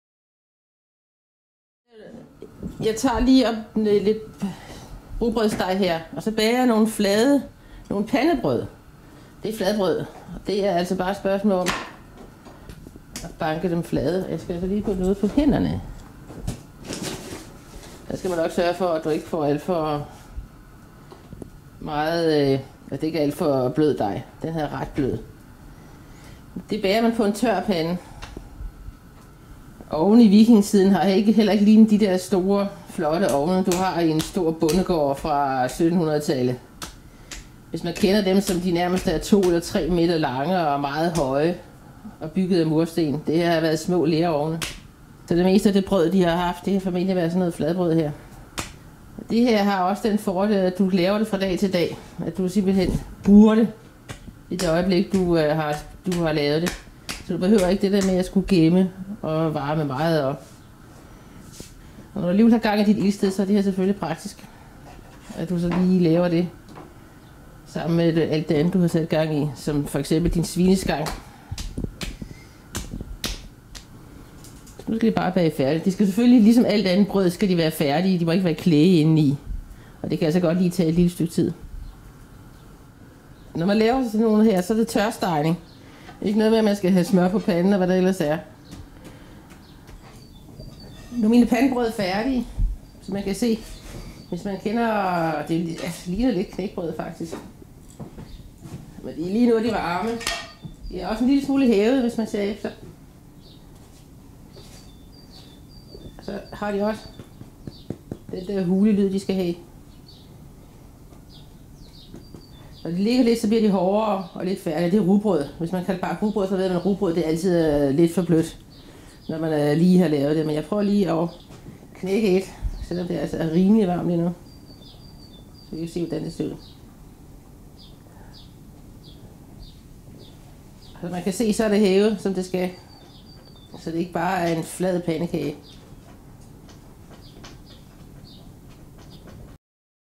Vikingemad: Pandebrød
Kokkepigen Inge-Mette viser hvordan man laver pandebrød af rugbrødsdej på rigtig vikinge manér.